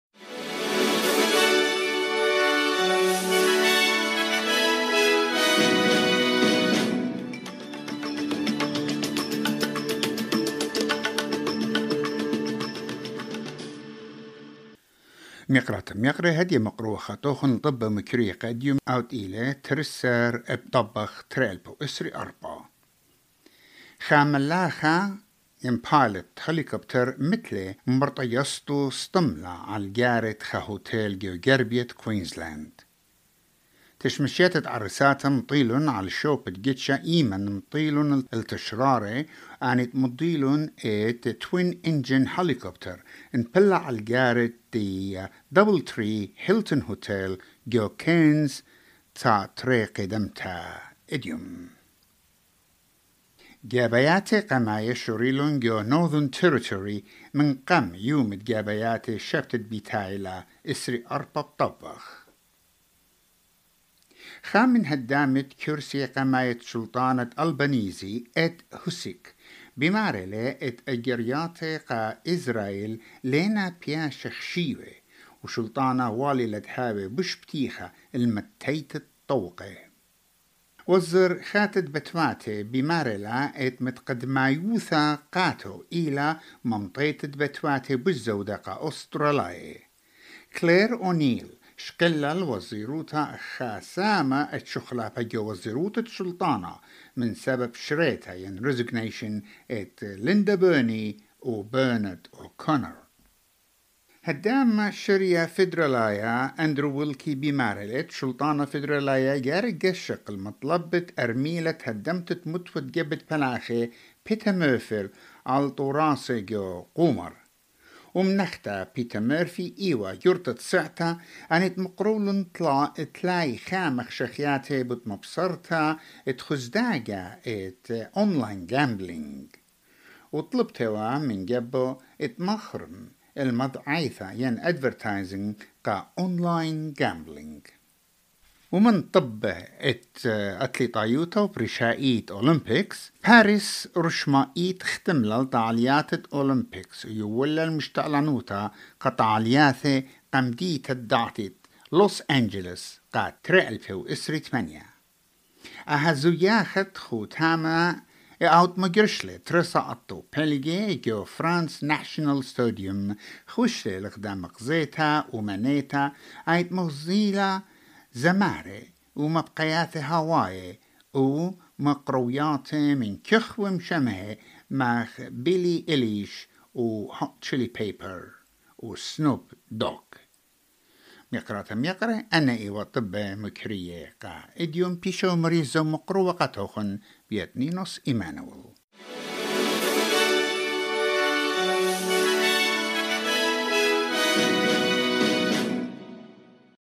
Newsflash: 12 August 2024 | SBS Assyrian
A pilot dead after a helicopter crashes into the roof of a hotel in Queensland; Early voting begins ahead of the Northern Territory election next week; And in sport, Top breakdancing judge praises Australian b-girl Raygun's kangaroo move for originality.